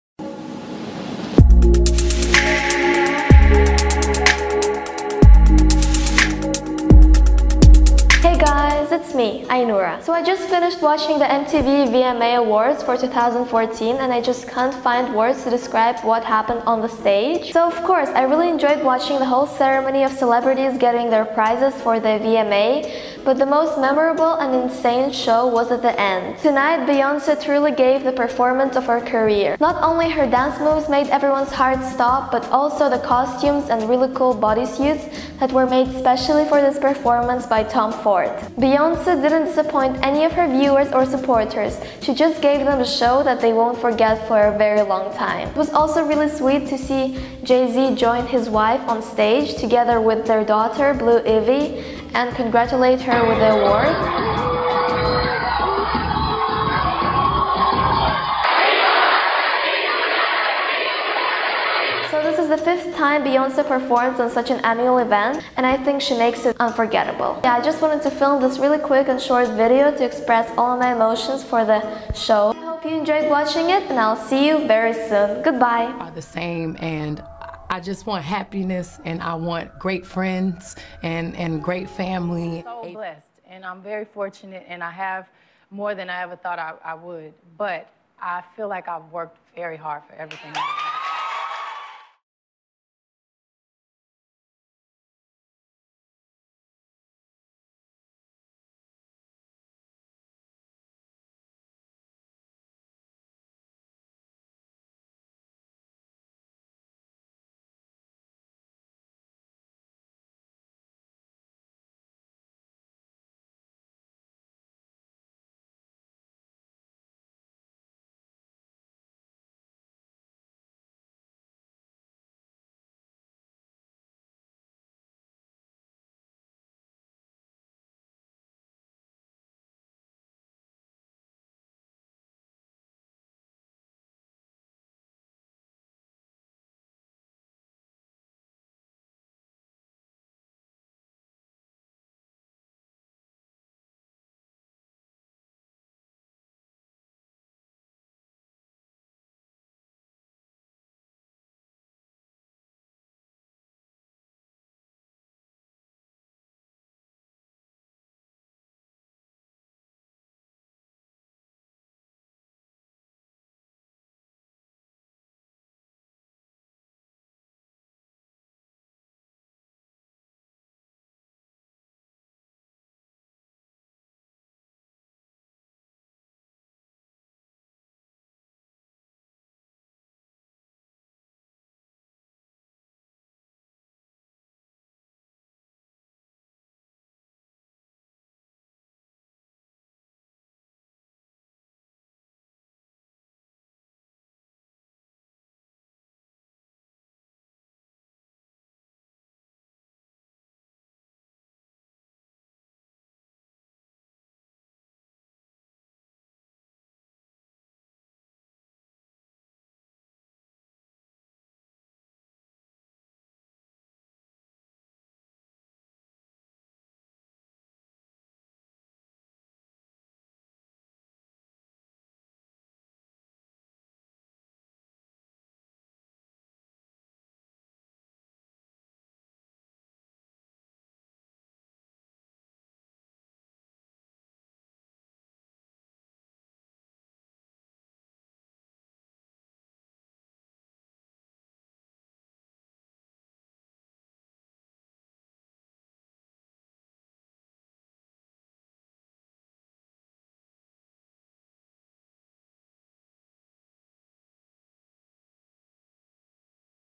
My opinion on Beyonce's Performance
Hi guys!

Sorry, I couldn't upload the actual performance because it is considered "illegal" on youtube.
You can watch the performance in good quality on mtv website
I just finished watching the Video Music awards and it was insane! I decided to film this short and quick video to express all my emotions! :)

Camera use: Canon Rebel T4i (with Sigma 30mm lens)